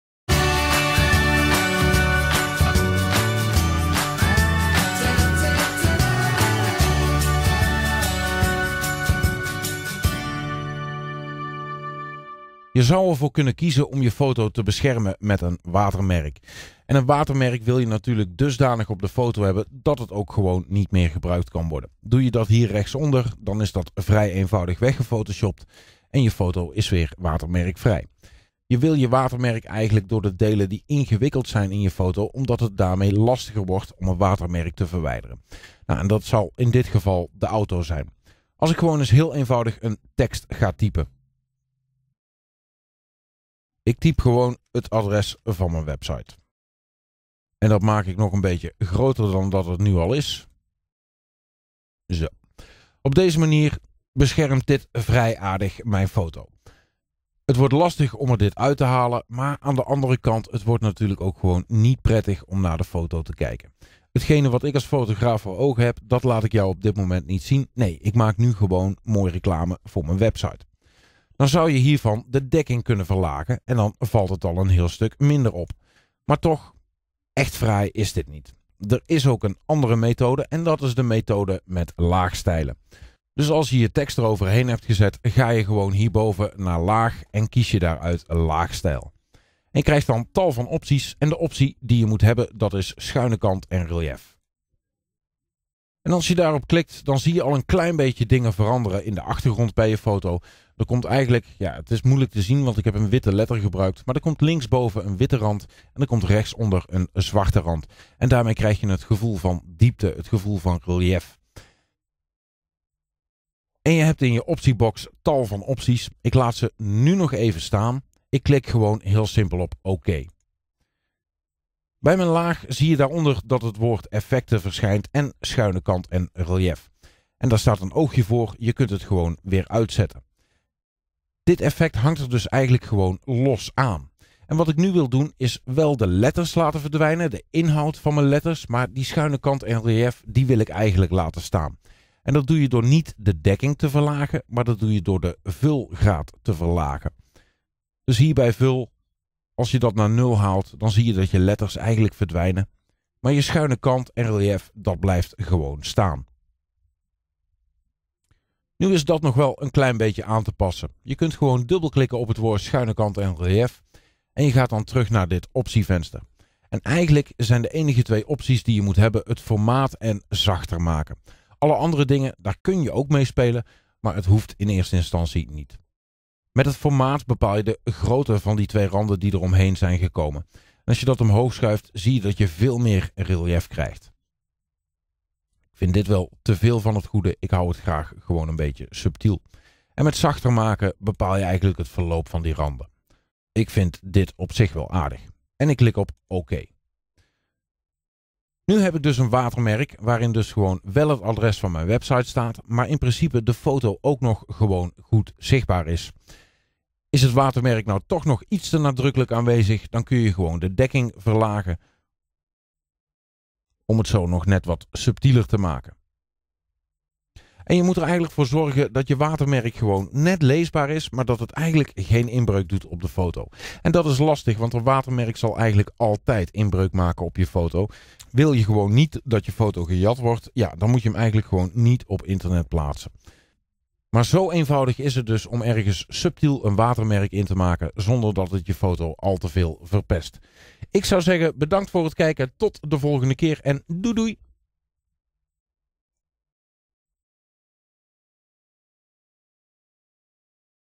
Subtiel watermerk met laagstijlen (Photoshop)
Een watermerk moet je foto beschermen. Maar een watermerk moet je foto niet overheersen. Een subtiel watermerk beschermt aan de ene kant je foto. Aan de andere kant zal het je foto ook voldoende kunnen beschermen tegen diefstal.

Een subtiel watermerkje wat er maar net een beetje op ligt, dat doe je niet door de dekking te verlagen. Met behulp van laagstijlen kun je namelijk iets veel mooiers maken.
Ik laat in deze film zien hoe.